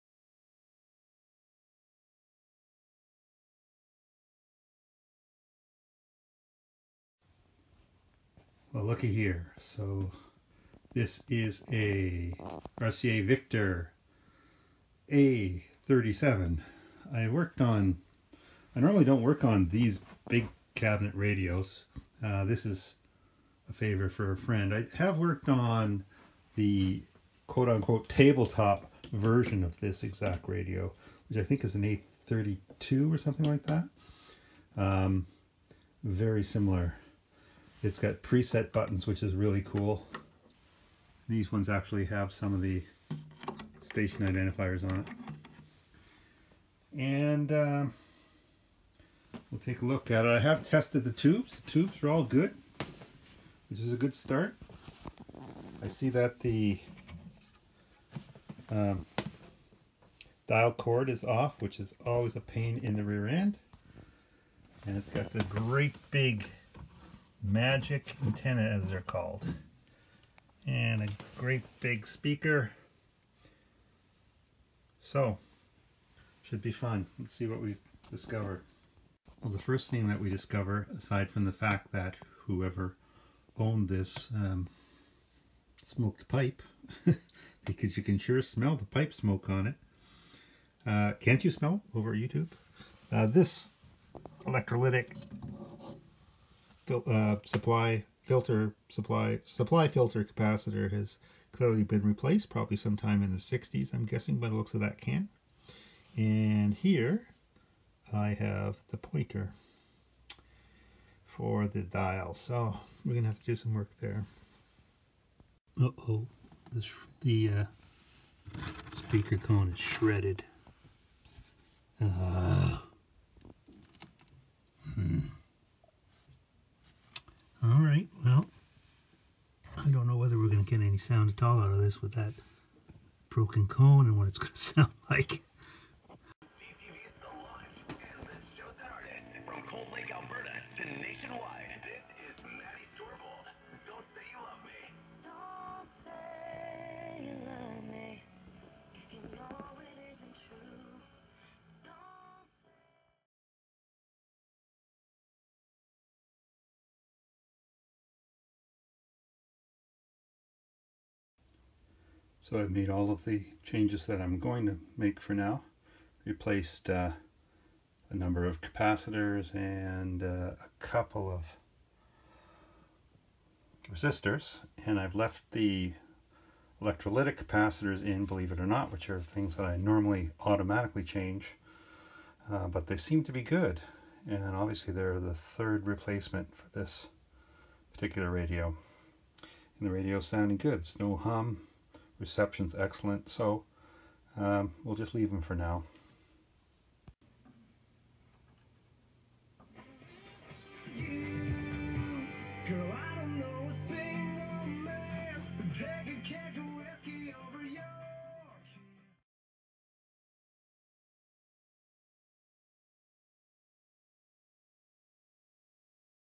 RCA Victor A37
Repairing a 1941 RCA Victor A37.